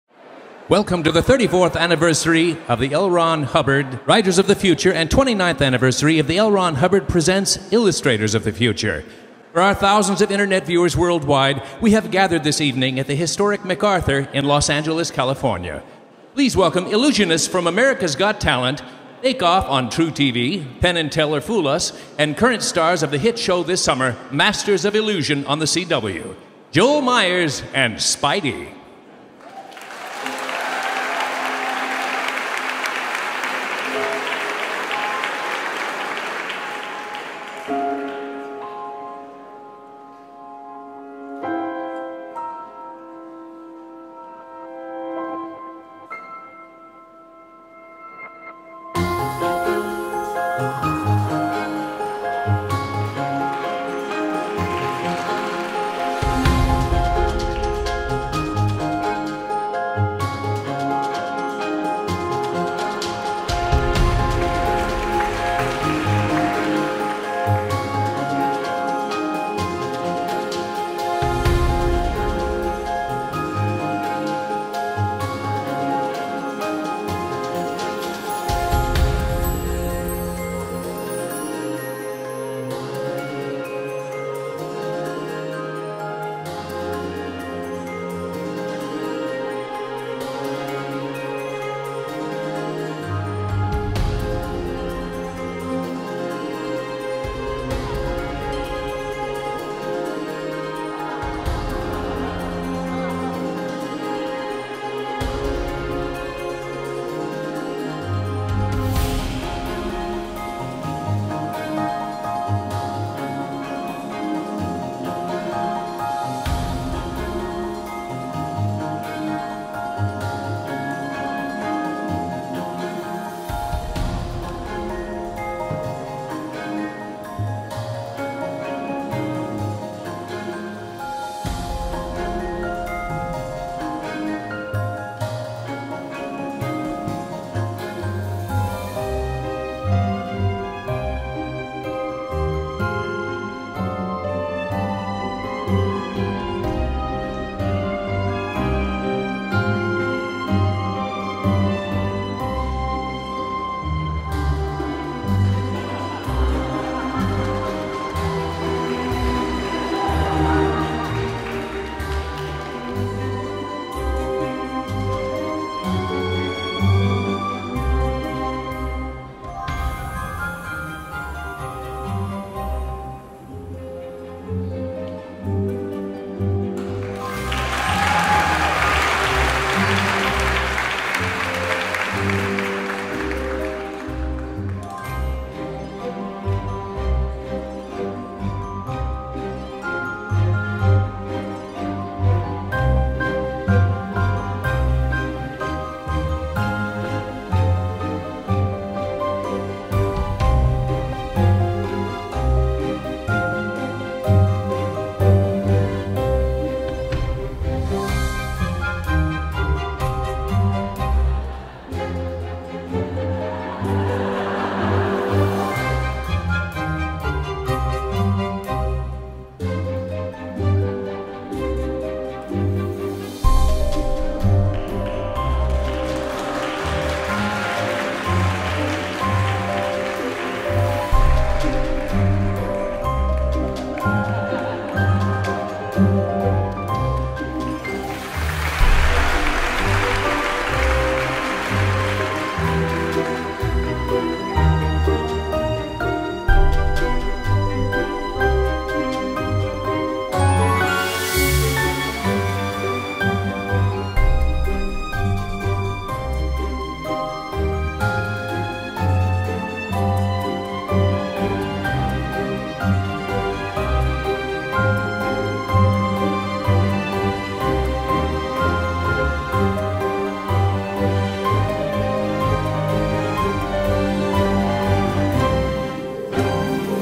Joel Meyers & Spidey - Writers of the Future Awards Show (Opener Full)
Joel Meyers & Spidey Headline the Writers & Illustrators of the Future Awards Show in Los Angeles! Special guest Jade PettyJohn! Full Opener.